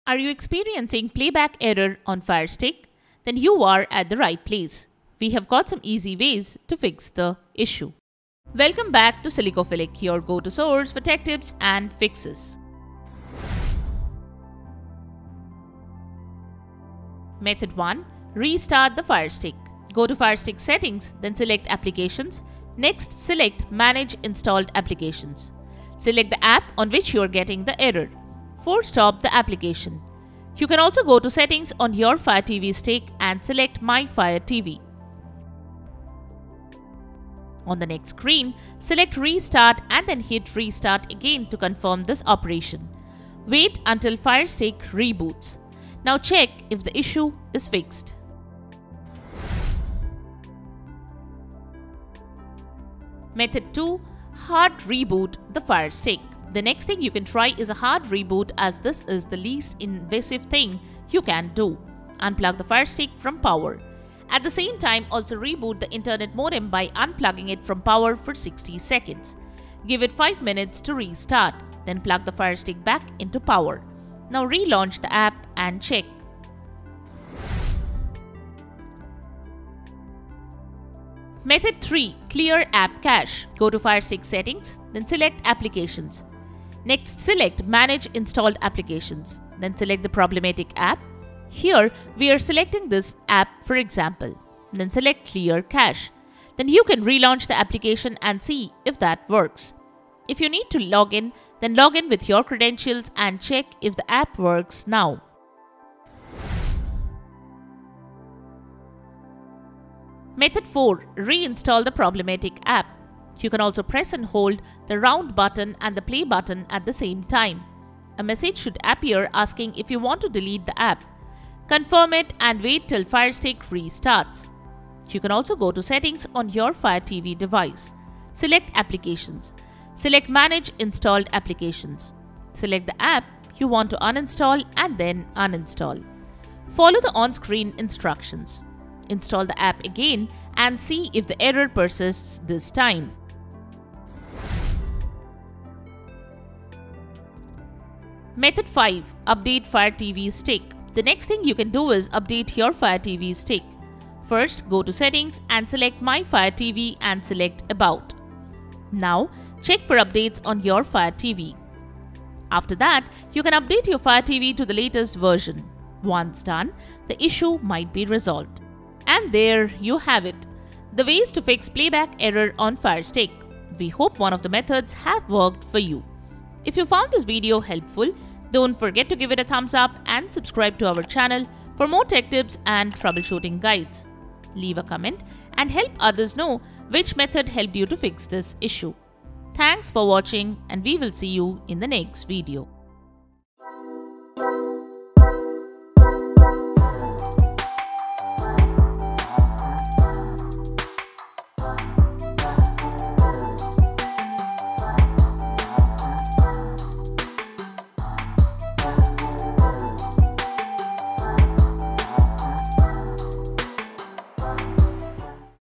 How to Solve Playback Error on Firestick | Step-by-Step Guide!
Playback errors on Firestick can be frustrating, but they're often solvable. Here are some common solutions to try:

Playback on Firestick refers to the ability of the Amazon Firestick device to stream or play media content such as movies, TV shows, music, or games. It involves the performance and stability of streaming apps like Netflix, Prime Video, Hulu, YouTube, Disney+, and others. However, various factors can impact the smoothness of playback on a Firestick device, leading to errors or interruptions.

Common Playback Issues on Firestick:
Buffering: Slow loading or frequent pausing during streaming, often caused by a weak or unstable internet connection.
Playback Error: This might appear as a message like "Something went wrong" or "Playback failed," often due to a software glitch, outdated app, or network issue.
App Crashes: The app you're using may suddenly close, preventing the content from playing.
Freezing or Lagging: The Firestick might become unresponsive during playback, resulting in frozen images or slow performance.
Causes of Playback Issues:
Network Issues: Slow or interrupted Wi-Fi connections are one of the most common reasons for playback errors.
App Issues: Outdated or buggy apps can also cause playback problems.
Firestick Performance: Over time, the Firestick can accumulate cache and data, slowing down its performance, which can affect playback.
Overheating: Extended use or insufficient ventilation can cause the Firestick to overheat, affecting its ability to perform tasks like playback.
Server Issues: Sometimes the issue is with the streaming service itself. Server downtimes or overloads can result in playback errors.

Timestamps:
00:00 Intro
00:15 Method 1
00:51 Method 2
01:19 Method 3
01:48 Method 4
02:26 Method 5